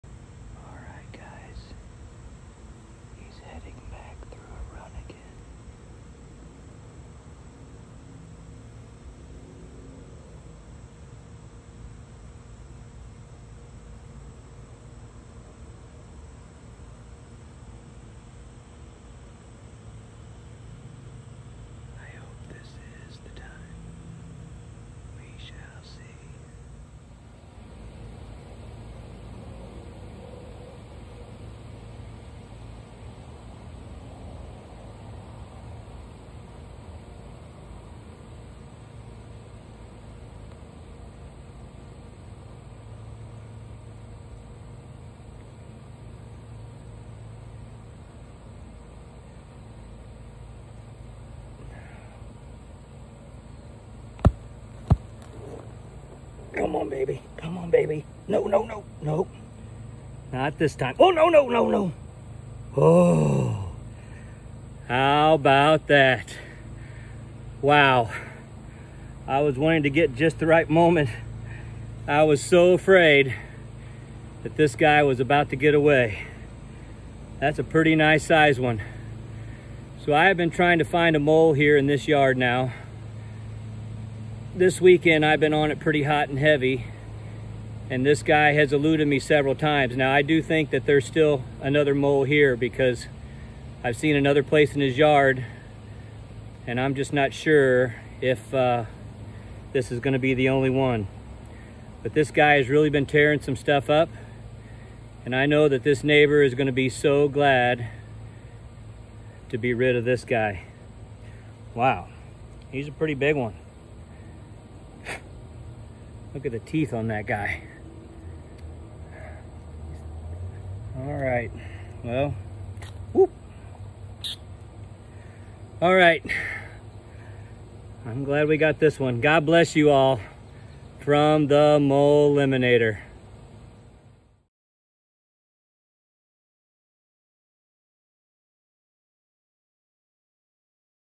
THE DAY OF RECKONING...Barehanded Mole Catching with the MOLiminator
THE DAY OF RECKONING...

My neighbor first told me about this mole last Saturday. He had set up a chair near the mole run and had just missed him earlier that morning trying to catch him with a shovel. He had done a lot of damage! Yesterday I watched him work twice and missed opportunities both times. Once he must have heard me and stopped working and the second time he was at a “T” in the tunnel and just got away. I walked all the runs down again and then checked them early this morning before church. This afternoon I saw where he was coming right back out of the woods heading through the yard. I started following the freshly worked up dirt and there he was! I had hunted this fat little fella hard. I think he may have a buddy in the same yard...we shall see! Until then I know this neighbor will be beaming! 🤠

Each of us should please our neighbors for their good, to build them up. Romans 15:2